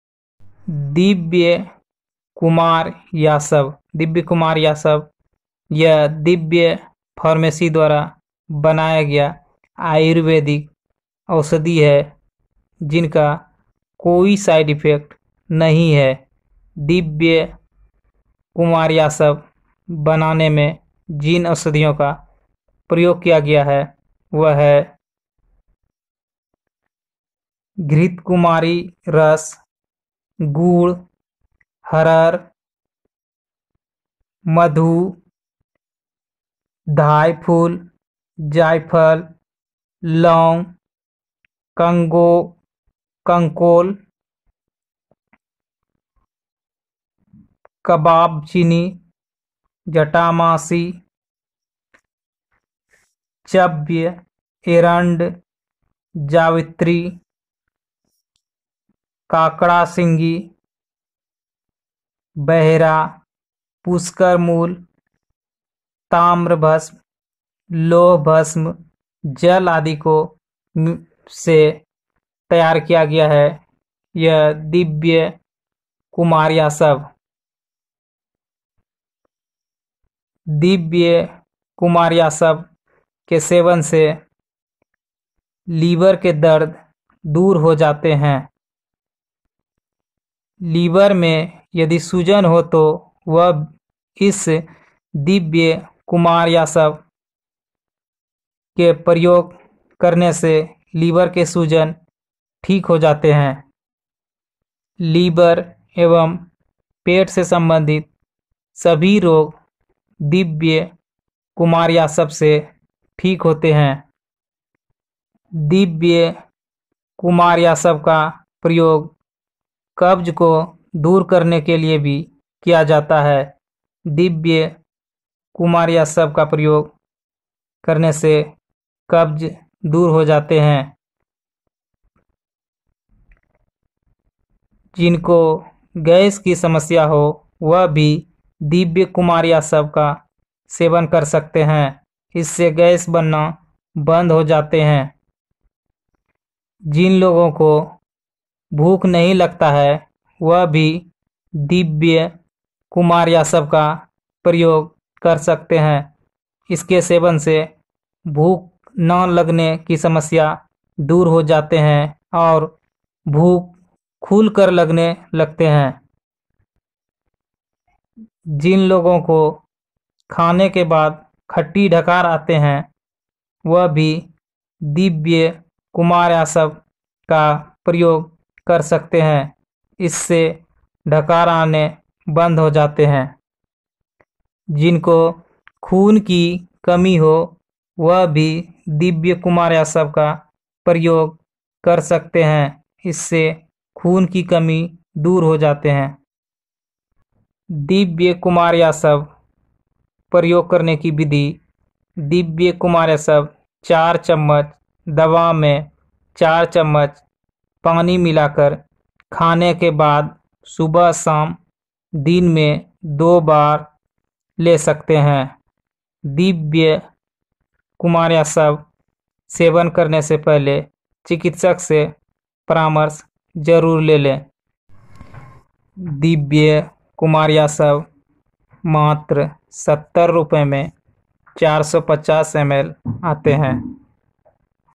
गैस,कब्ज यकृत-शूल,यकृत-शोथ,पेट दर्द,लीवर सूजन,लीवर रोग,पेट रोग,आदि में लाभकारी है दिव्य कुमार्यासव
स्वास्थ्य से संबंधित जानकारी